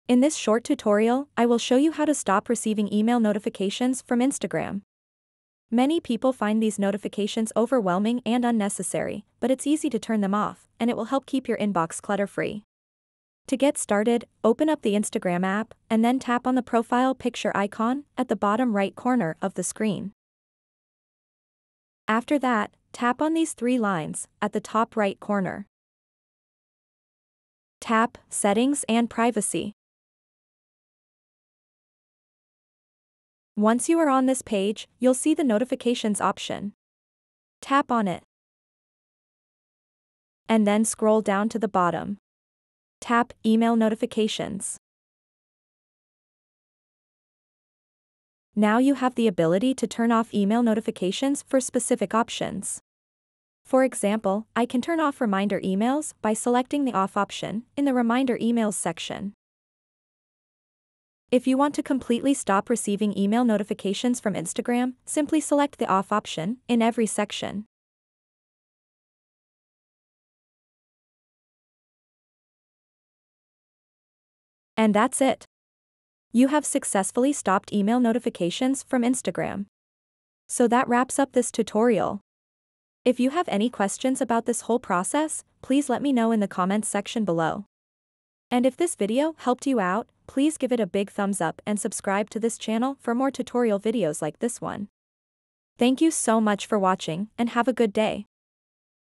How To Stop Email Notifications From Instagram
In this short tutorial, we will show you how to stop email notifications from Instagram. If you're tired of receiving too many emails from Instagram, you can easily turn off these notifications so that you can enjoy a clutter-free inbox.